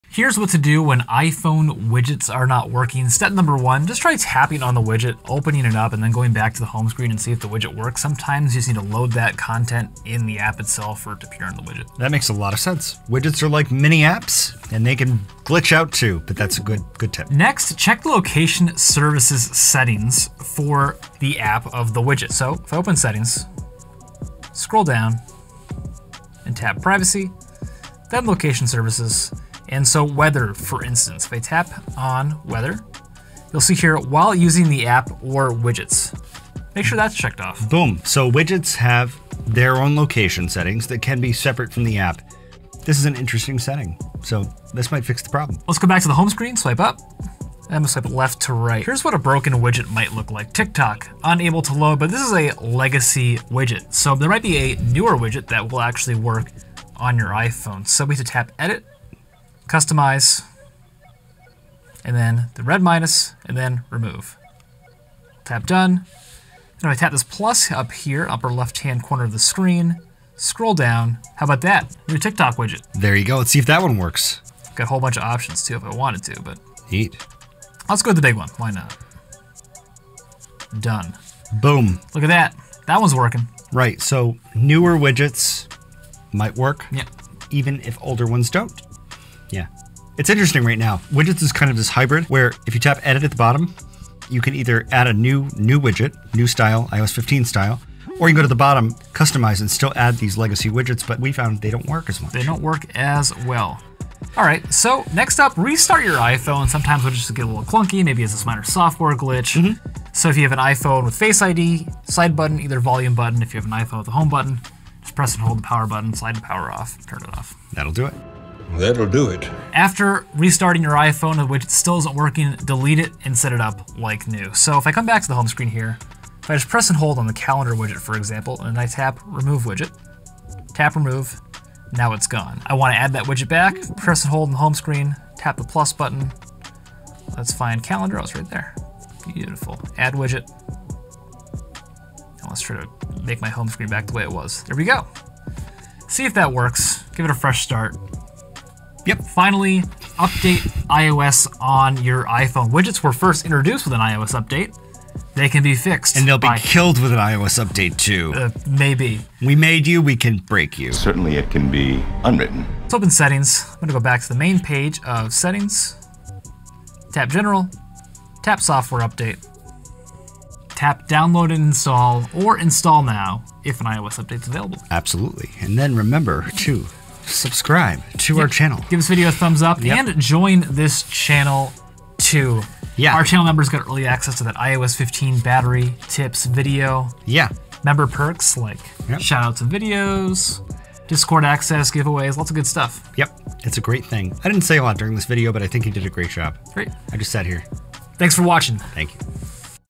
iPhone Widgets Not Working? Here's The Fix!
David and David explain how to fix the problem when your iPhone widgets are not working. Widgets are little "mini-apps" that display a little bit of information from an app directly on the Home screen. While informative and a fun way to customize your iPhone, they can sometimes be clunky!

1. Introduction [0:00]
2. Tap On The Widget [0:03]
3. Check Location Services [0:17]
4. Try Installing A Newer Widget [0:45]
5. Restart Your iPhone [1:45]
6. Delete And Reinstall The Widget [1:59]
7. Update iOS On Your iPhone [2:29]